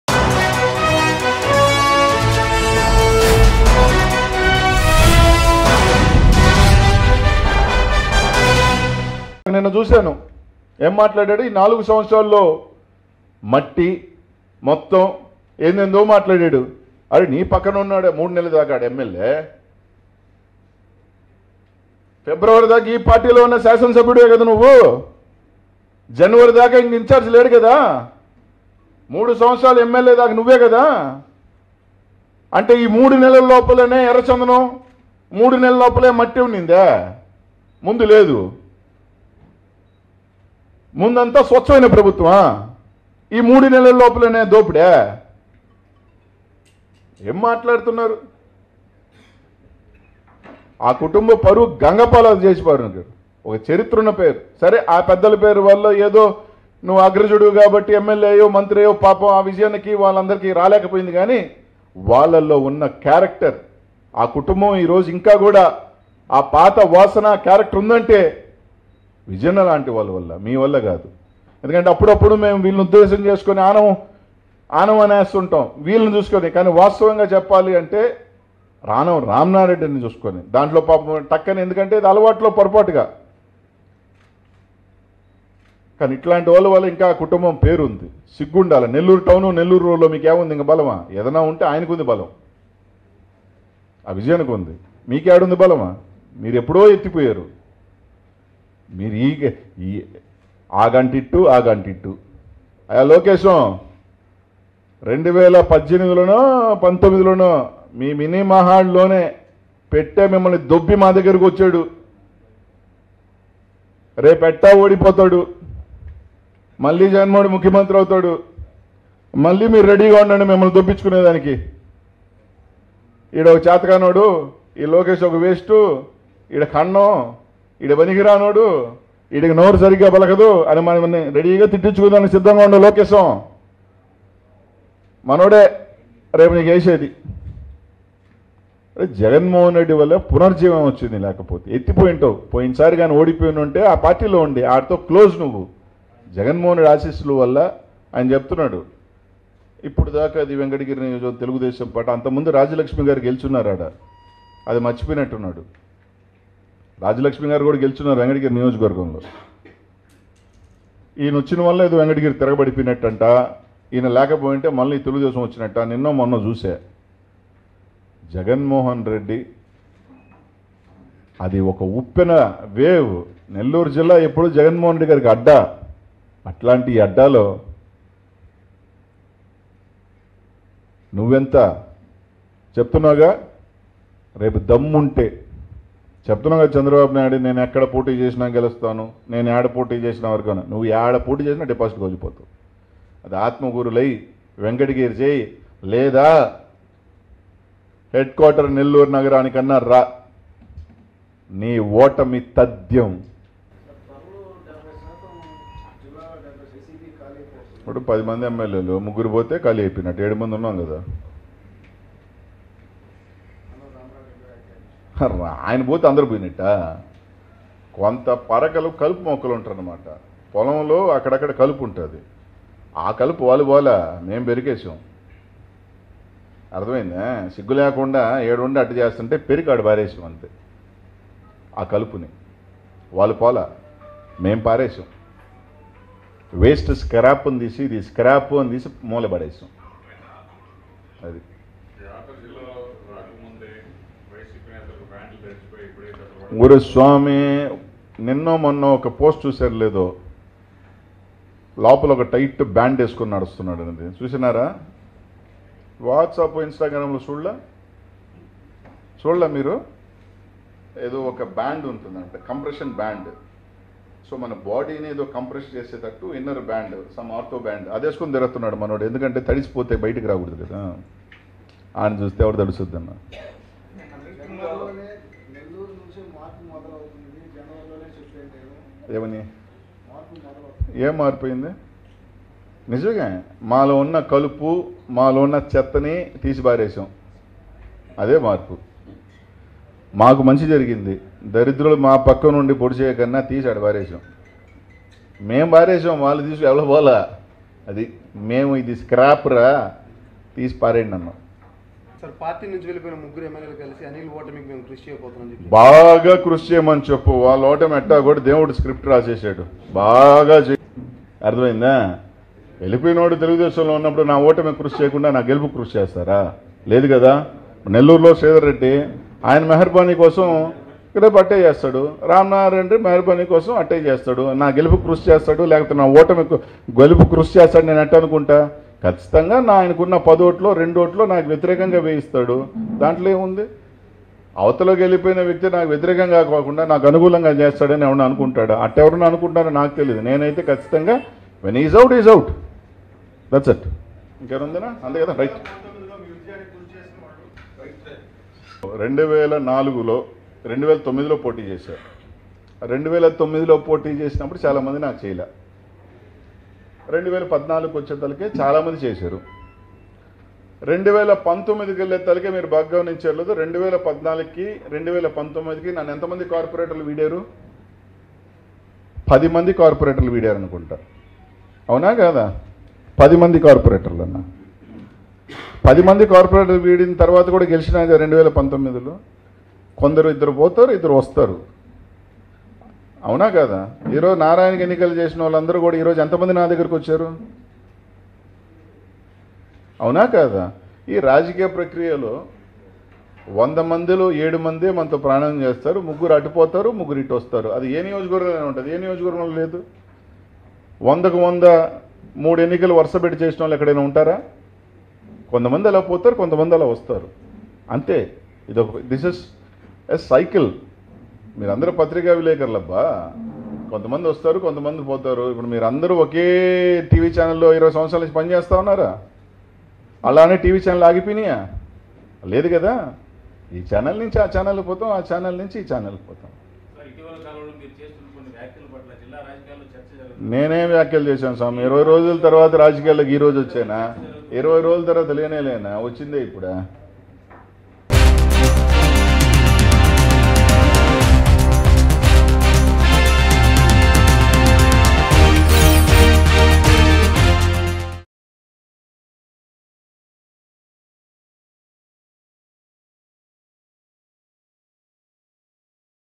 Anil Kumar Yadav Powerful Words About CM YS Jagan In Press Meet | YCP Latest News | Mango News
Anil Kumar Yadav Powerful Words About CM YS Jagan In Press Meet.